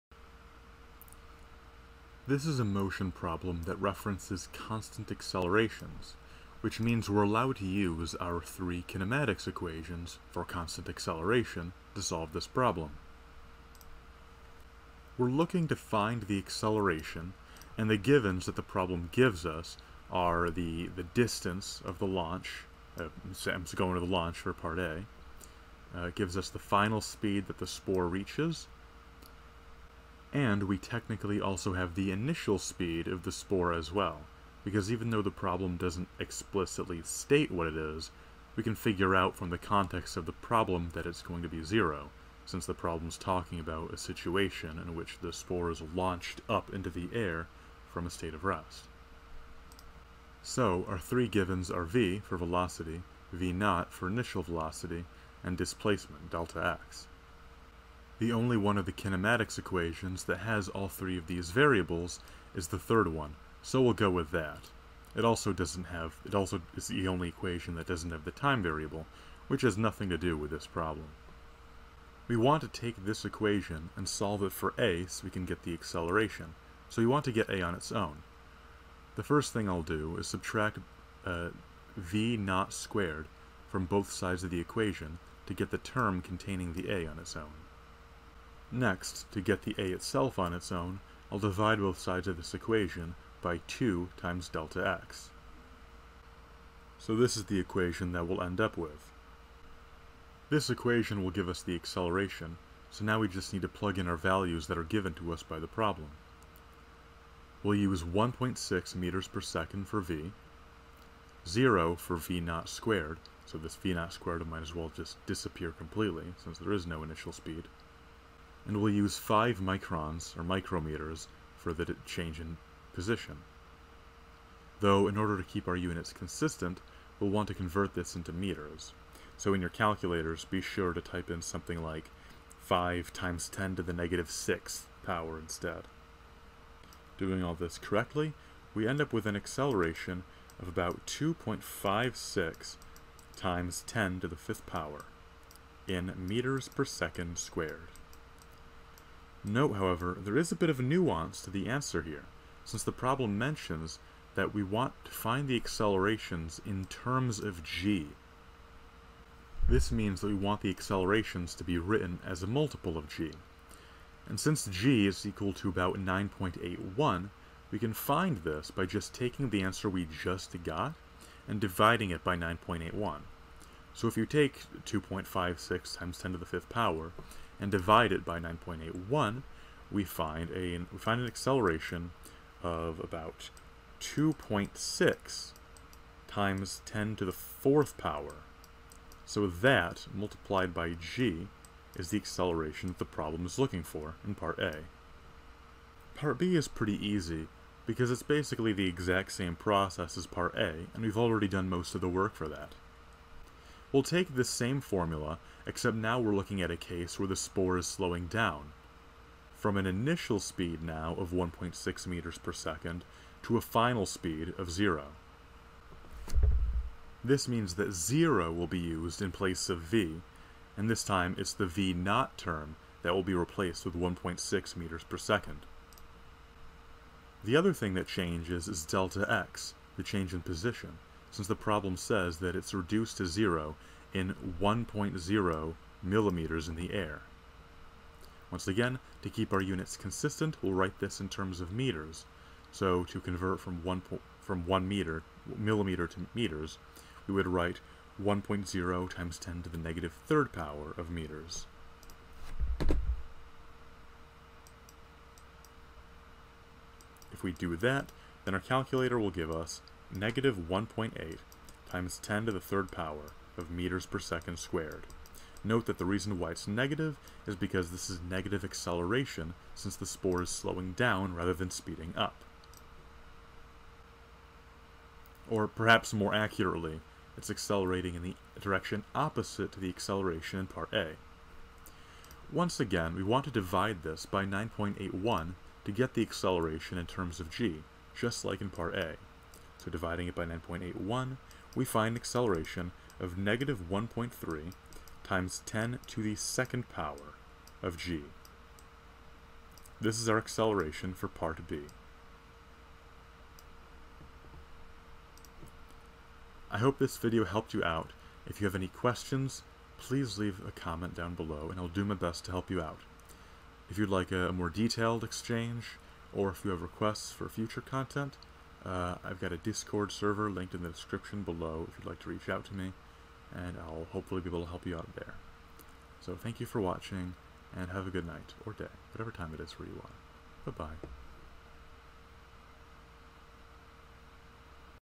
Certain mushrooms launch their spores by a catapult mechanism. As water condenses from the air onto
Certain mushrooms launch their spores by a catapult mechanism. As water condenses from the air onto a spore that is attached to the mushroom, a drop grows on one side of the spore and a film grows on the other side. The spore is bent over by the drop’s weight, but when the film reaches the drop, the drop’s water suddenly spreads into the film and the spore springs upward so rapidly that it is slung off into the air. Typically, the spore reaches a speed of 1.6 m/s in a 5.0 mm launch; its speed is then reduced to zero in 1.0 mm by the air. Using those data and assuming constant accelerations, find the acceleration in terms of g during (a) the launch and (b) the speed reduction.

Even a small amount of money can add up and support me immensely!